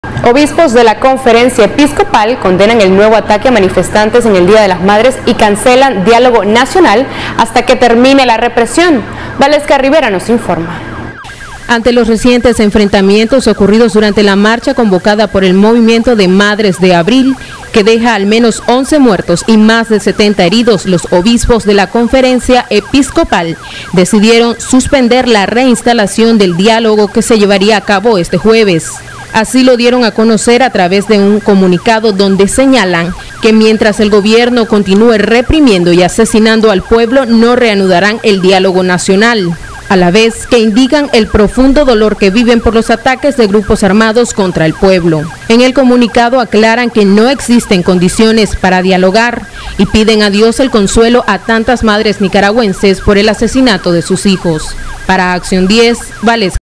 Obispos condenan ataque a manifestación de madres
Suspenden la reinstauración del diálogo hasta que el Gobierno deje estas prácticas violentas. Y piden a Dios el consuelo para las madres nicaraguenses por el asesinato de sus hijos.